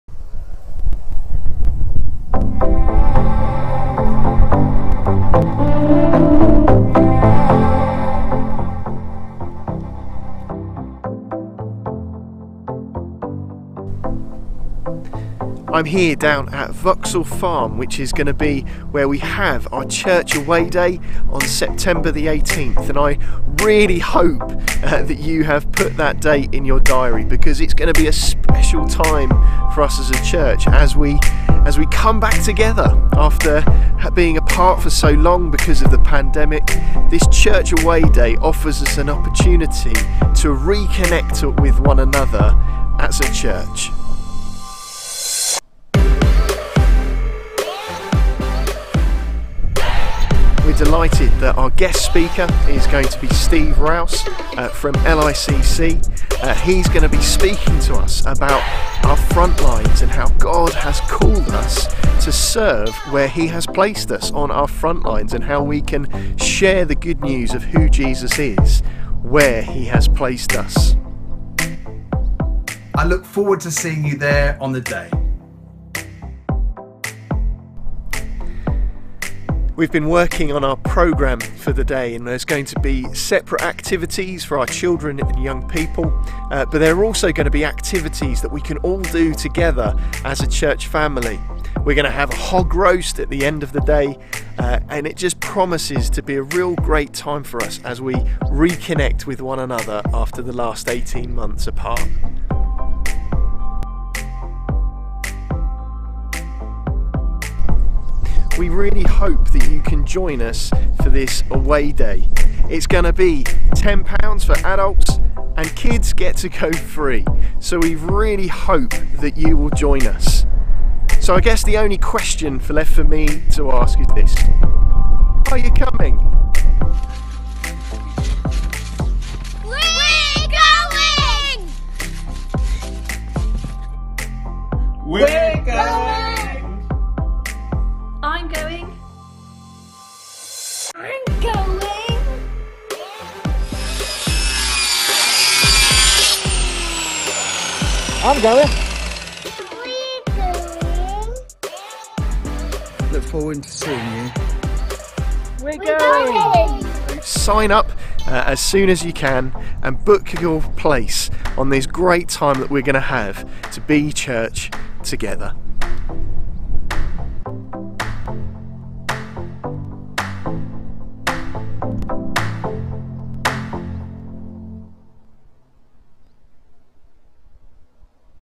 Away Day - 18th September 2021
On the 18th September we have our church away day at Vauxhall Farm where Steve Rouse from The London Institute for Contemporary Christianity will be speaking to us in the morning as he launches our Autumn focus "Life on the Frontline". We all have a frontline – a place where we do life or work, encountering people who don’t know Jesus.

Sign-up to be a part of this vital moment in the life of our church!
A full program has been planned with activities for Young and Old alike.

We are excited that Steve Price will be joining us for the evening of our Church Away Day with his well-honed blend of comedy,  magic and ministry... adding to our day of laughter and enjoyment.

Steve is the winner of The Magic Circle’s Originality Prize and will give our evening message a real twist that you will never forget.